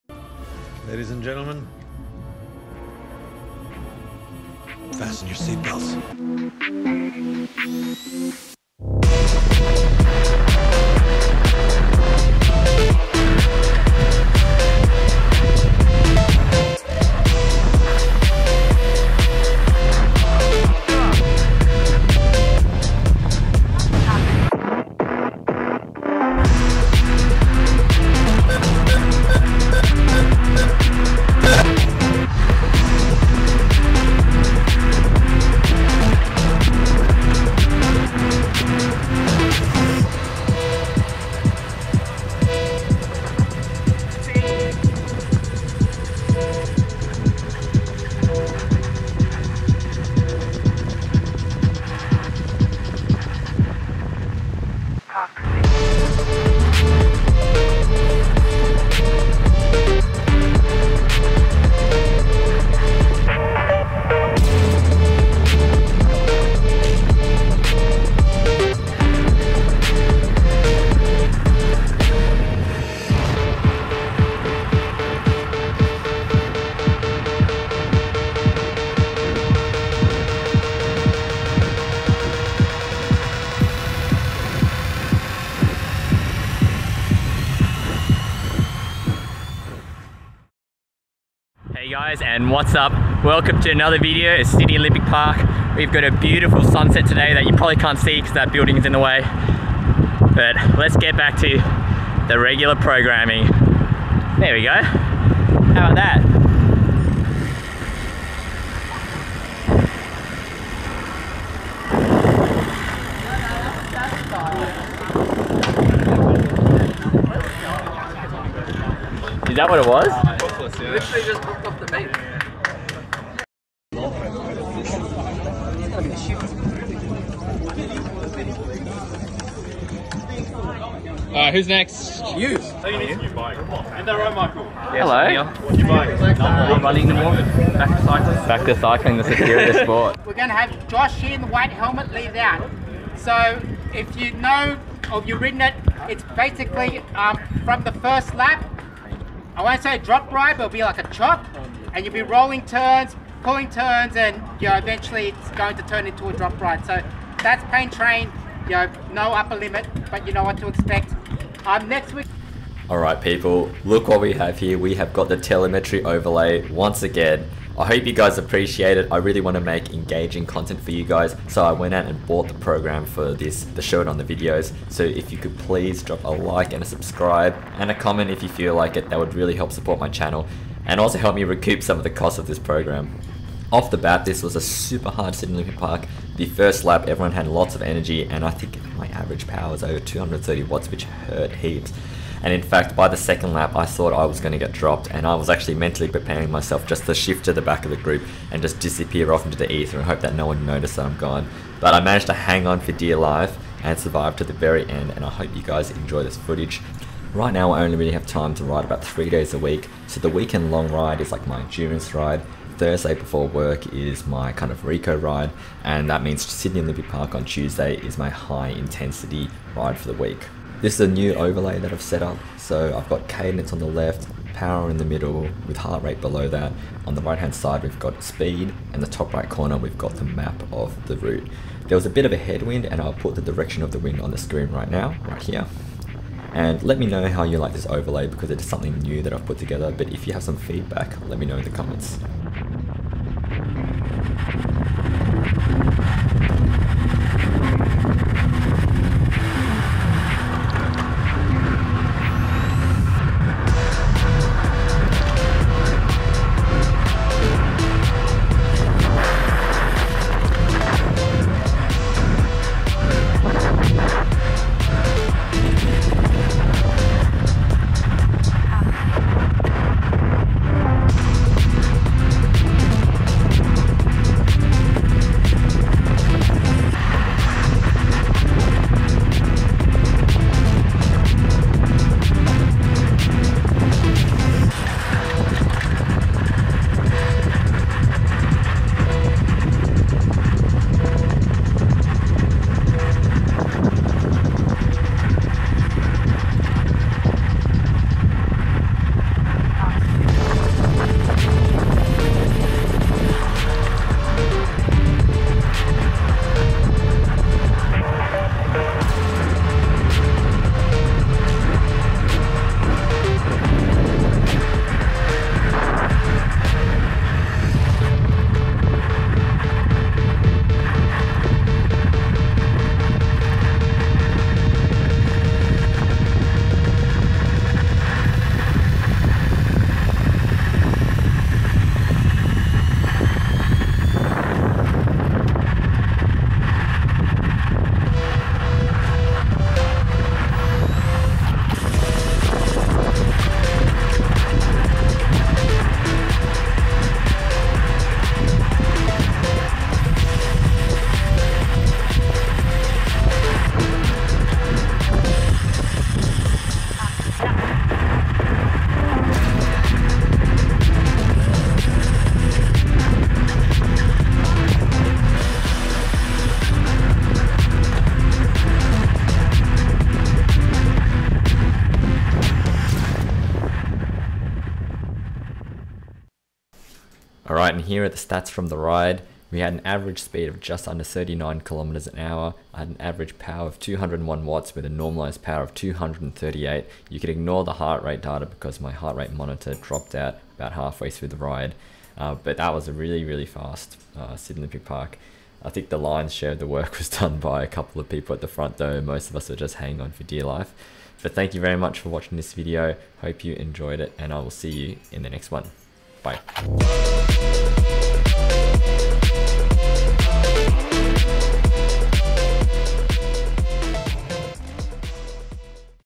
Hung, drawn and quartered at Sydney Olympic Park Pain Train
Telemetry overlay is BACK. We have all the juicy data back on screen that you guys know and love. Cadence, speed, heart rate and power. This was a fantastic week to bring back the overlay, with ripping corners, people dropping watt bombs and a sizable group at Pain Train. Hope you guys enjoy the video as well as the introduction was made and inspired by Nem who requested 'more memes' in the vids, this one is for you mate.